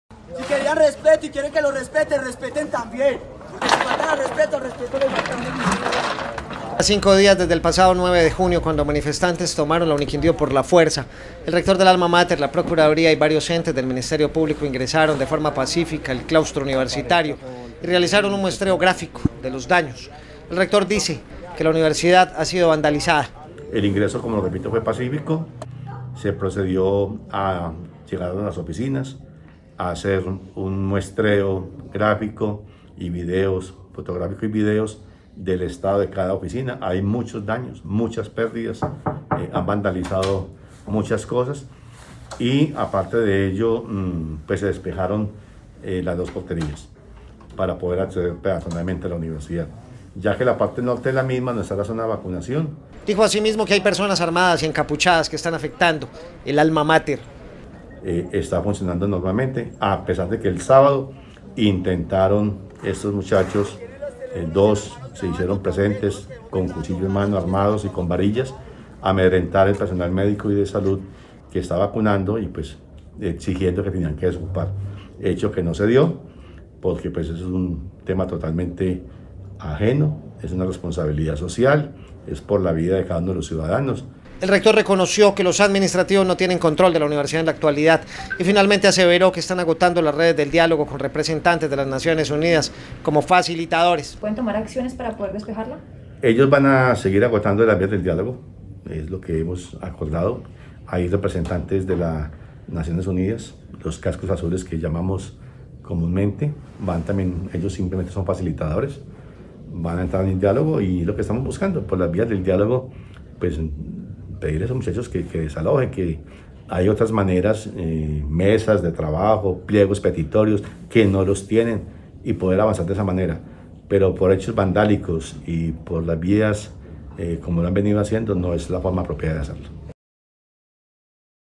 5 días desde la toma, de la Universidad pública del Quindío
Tras cinco días desde el pasado 9 de mayo cuando manifestantes  tomaron  la Uniquindio, el rector de la alma mater , la procuraduría y varios entes del ministerio público, ingresaron de forma pacífica a la Universidad y realizaron  un muestreo grafico de los daños, el rector dice, que La universidad ha sido Vandalizada.